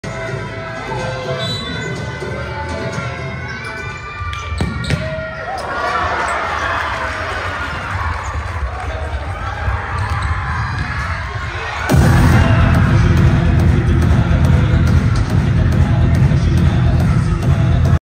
I got your back mate! 🧤🤝
Best feeling as a goalkeeper is a goal being scored by your teammates after powerful save 🧤🔥🤠.  Borås aik futsal goalkeeper Camil Altun with a brilliant save and a lovely lobshot Which his teammates nicely converted into the most easy goal you can score 😆🤝
Borås aik futsal vs Norrköping futsal, SFL